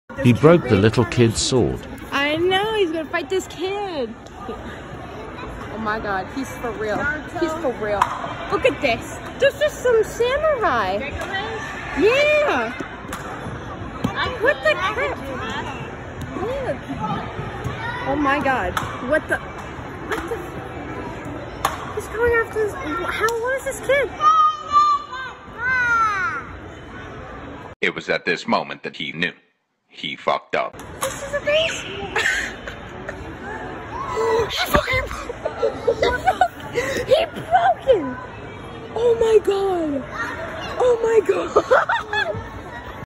"Abort Abort Leave The Area Now!!" -Swording fighting a little kid- ((GONE WRONG))
This is why we don't let our friend fight little kids with swords.